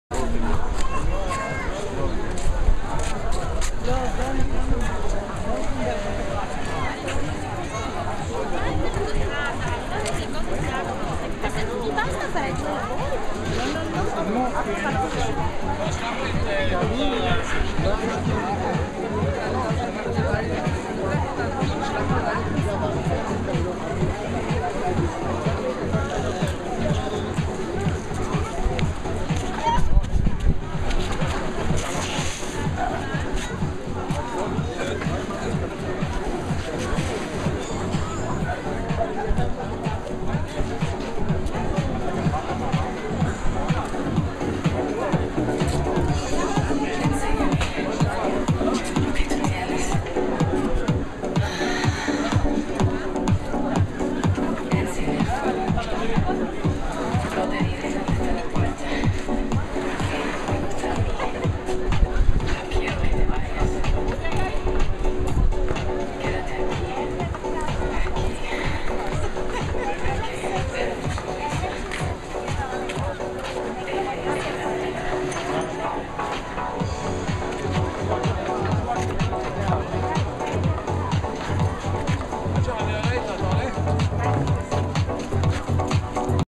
אורי ואלון בחוף הרצליה
אורי ואלון בקרב קראטה מרהיב בחוף הרצליה